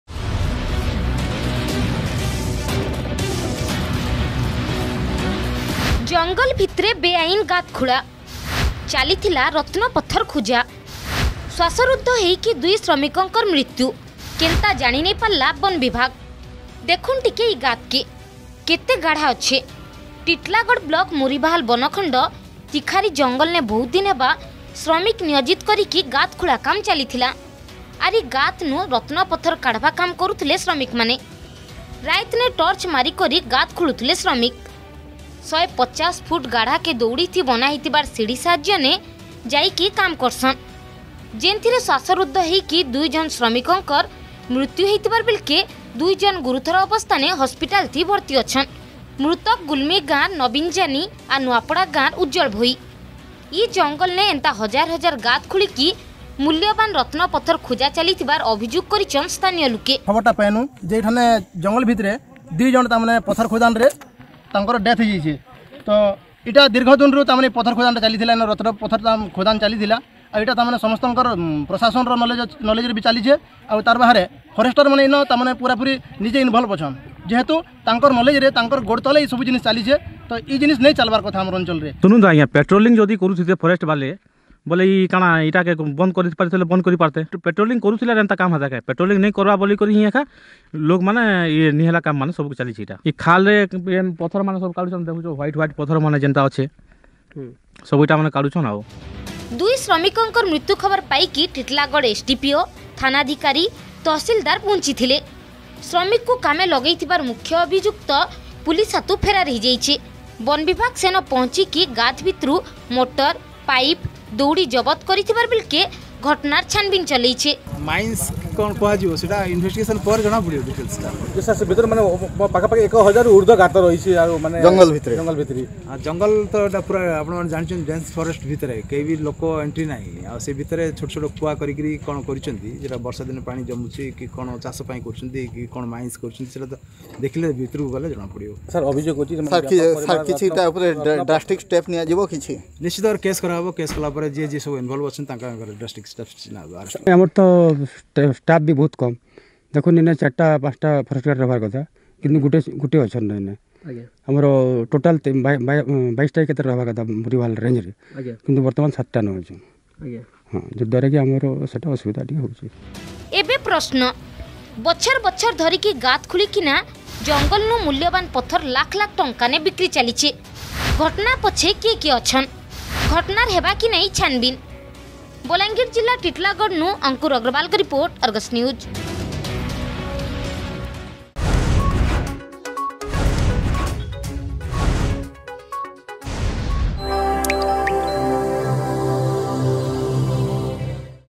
ଜଙ୍ଗଲ ଭିତରେ ବେଆଇନ୍ ଗାତ ଖୋଳା
ଟିଟିଲାଗଡ ବ୍ଲକ ମୁରିବାହାଲ ବନଖଣ୍ଡ ତିଖାରି ଜଙ୍ଗଲନେ ବହୁତ୍ ଦିନ୍ ହେବା ବେଆଇନ୍ ଗାତ ଖୋଳା ଚାଲିଥିଲା । ୧୫୦ ଫୁଟ ଗାଢାକେ ଦଉଡ଼ି ଥି ବନାହେଇଥଇବାର୍ ସିଢି ସାହାଯ୍ୟନେ ଯାଇକି କାମ କରୁଥିଲେ ଶ୍ରମିକ। ଯେନଥିରେ ଶ୍ଵାସରୁଦ୍ଧ ହେଇକି ଦୁଇ ଝନ୍ ଶ୍ରମିକଙ୍କର୍ ମୃତ୍ୟୁ ହେଇଥିବାର୍ ବେଳକେ ଦୁଇ ଝନ ଗୁରୁତର ଅଛନ୍ ।